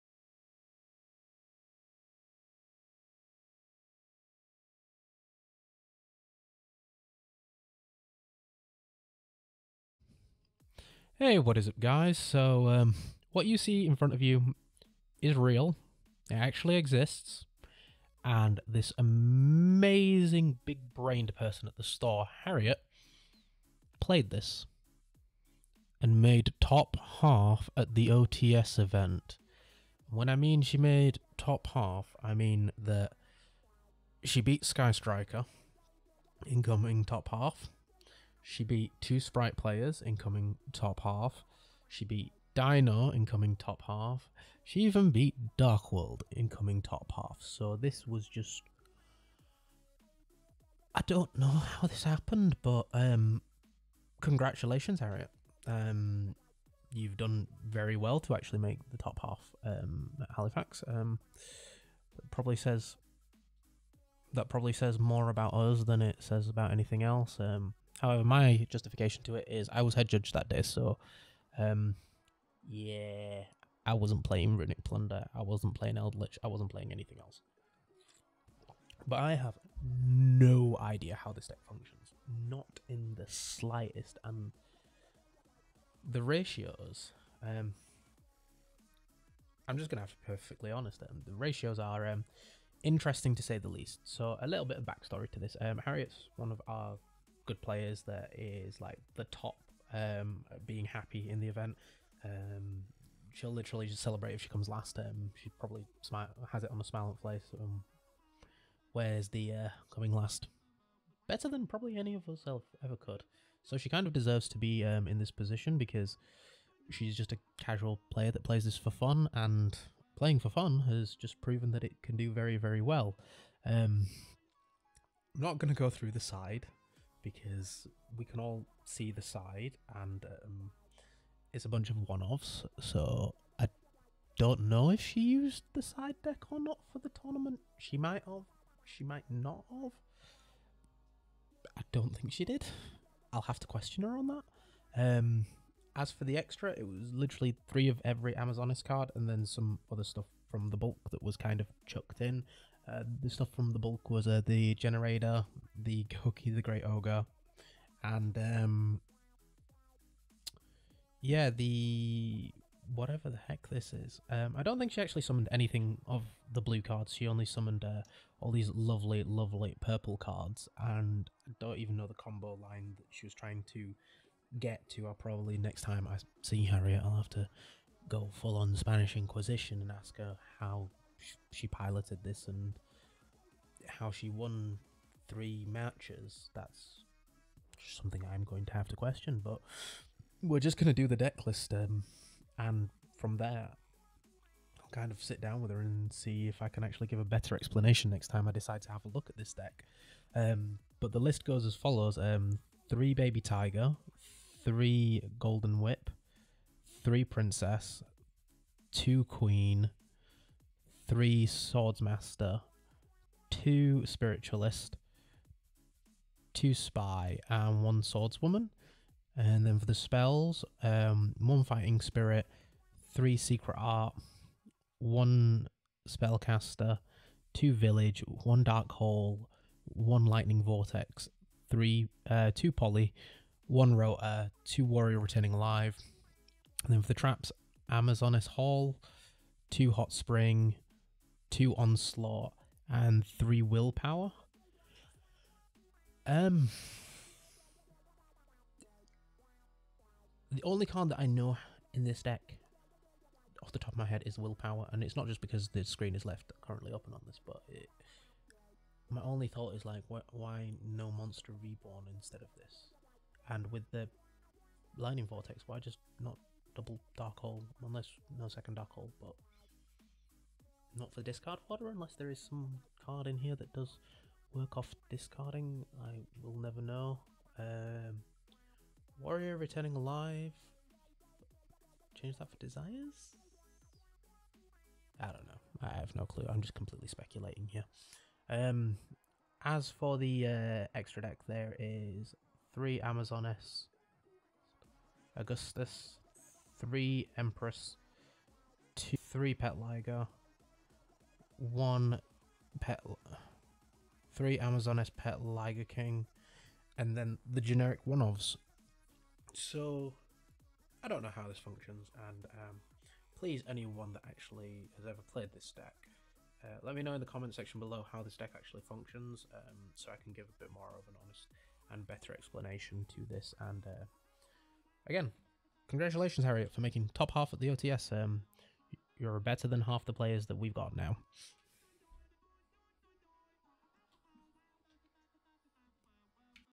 PURE - AMAZONIS AUGUST DECK 2023
Sorry about the audio still having teething problems with making videos.

Don't take everything in this deck as gospel I HAVE ONLY TESTED IN PERSON AGAINST VIRUS. DECK AND THAT SUCKED TO CALLING SPELL. other matches were fine just need more practice on the piolets part